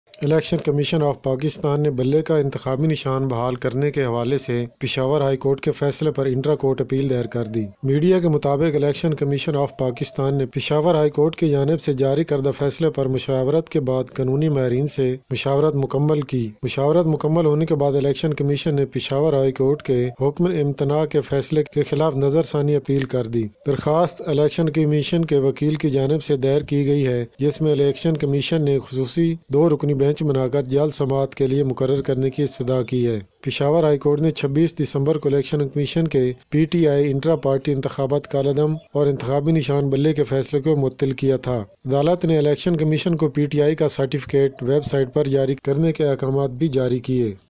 Election Commission Gave Big Shock to PTI | BAT Symbol For PTI | Breaking News
jhang
live

Peshawar High Court, Barrister Gohar, Pti Bat Symbol, Pti Chairman Imran Khan, Elections in Pakistan, Elections 2024, Election Commission,